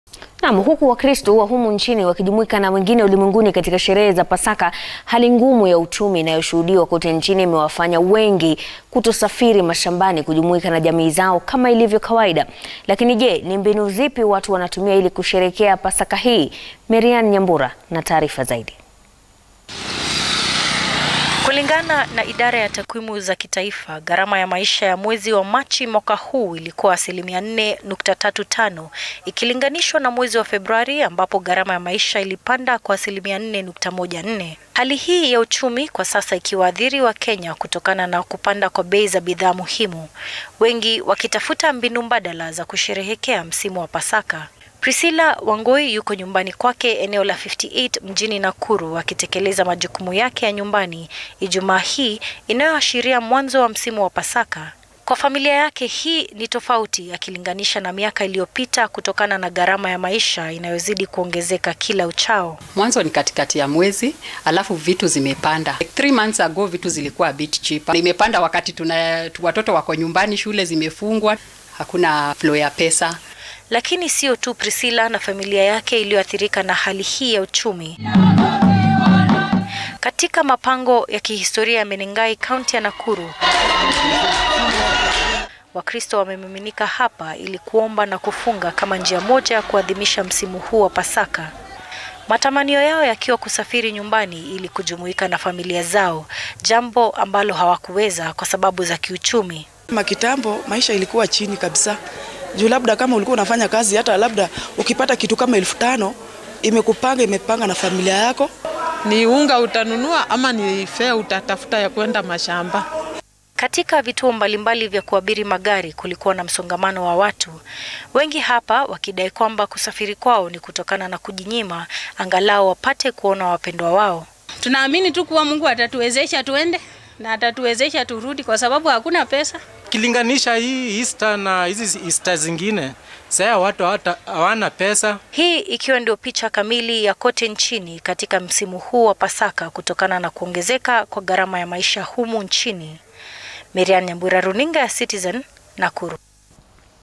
Hali ya uchumi yawaathiri wakenya
Huku wakristo wa humu nchini wakijumuika na wengine ulimwenguni katika sherehe za pasaka,hali ngumu ya uchumi inayoshuhudiwa kote nchini imewafanya wengi kutosafiri mashambani kujumuika na jamii zao kama ilivyo ada. Lakini je ni mbinu zipi watu wanatumia ili kusherehekea pasaka hii?